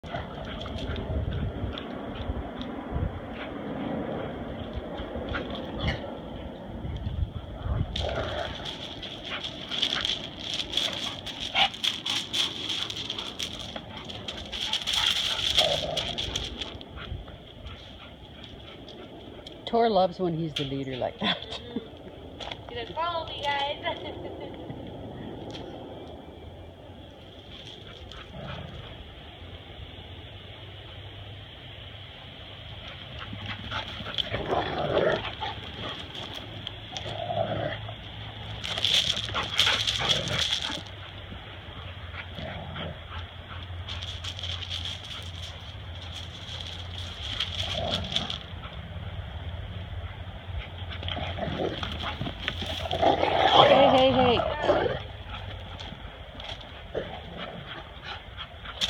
Catch me if you can
Tor loves to be chased and has become good at twisting, turning and avoiding capture. With Tobi in hot pursuit, and Ziva running interference, it's a great lunch-time break for the dogs